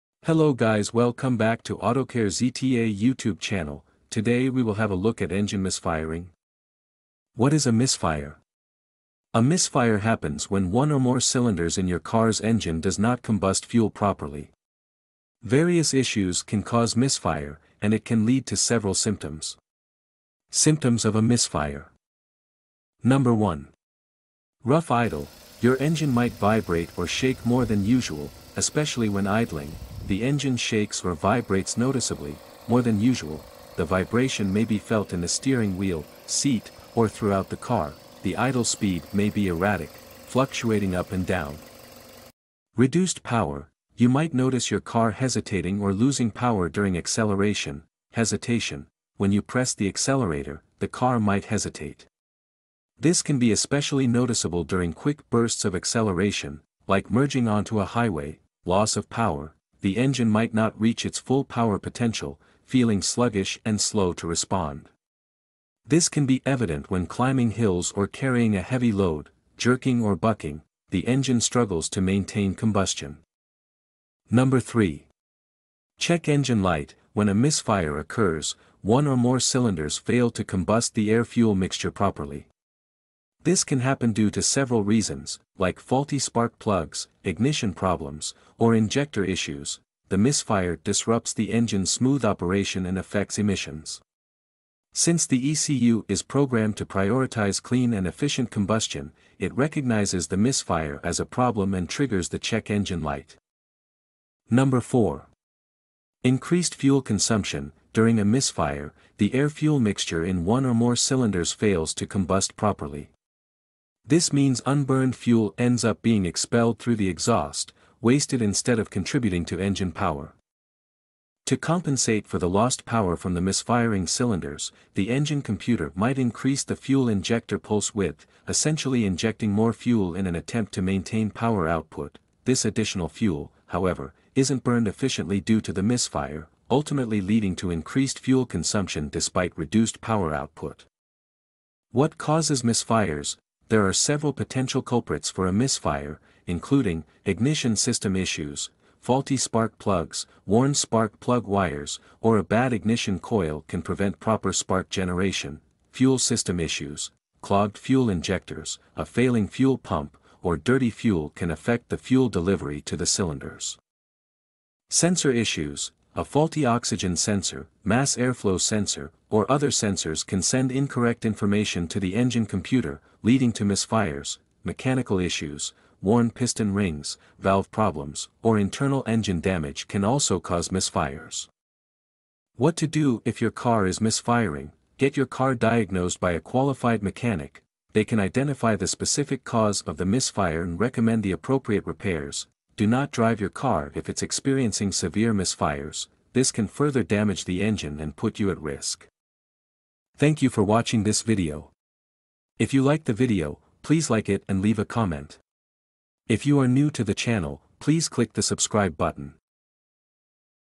4 Top Misfire Symptoms You Need to Know
Engine Misfire:
• What it is: An engine misfire occurs when one or more cylinders isn't burning fuel efficiently. This disrupts the smooth running of the engine.
• Symptoms:
o The car jerks or vibrates abnormally.
o You experience reduced power or sluggish acceleration.
o The engine might run rough, especially when idling.
o You may hear unusual noises like sputtering or popping from the exhaust.
o The "check engine" light might illuminate on your dashboard.
• Causes: There are several potential causes, including:
o Faulty spark plugs: These ignite the air-fuel mixture in the cylinder. Worn or damaged spark plugs can cause weak or mistimed sparks.
o Clogged fuel injectors: These deliver fuel to the cylinders. Clogged injectors can prevent enough fuel from entering for proper combustion.
o Ignition coil problems: The coil supplies the high voltage needed for the spark plugs. A failing coil might not deliver a strong enough spark.
o Other issues: Problems with the air intake system, sensors, or even engine wear can also contribute to misfires.